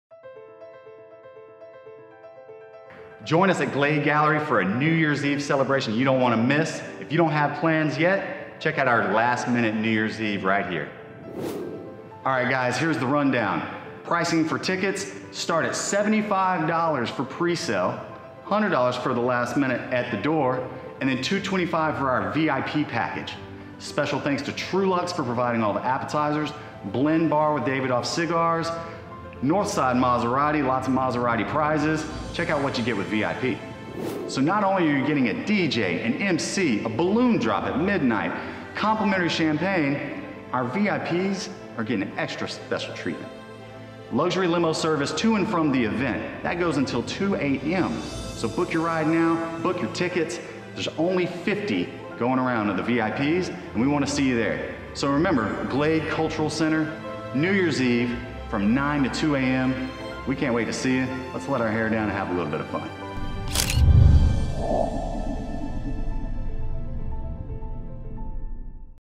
Last Minute NYE - Promo
Promo video for "Last Minute NYE" Party at Glade Gallery in The Woodlands. Hosted by Ross Flurry and Glade Arts Foundation.